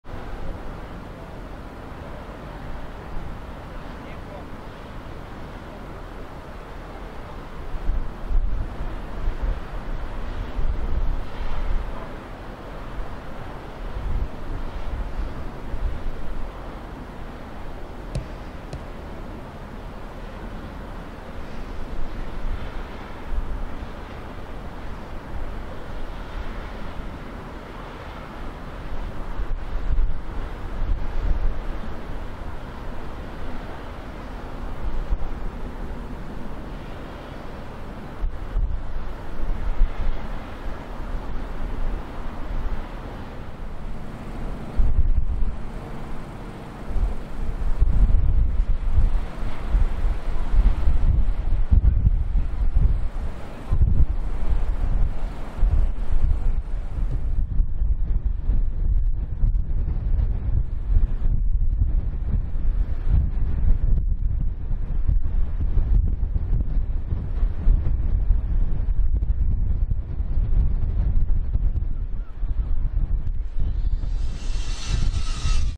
Ro Ro Ferry Bore Song arriving at Liverpool.
Arriving Thursday afternoon on the 17th of April 2024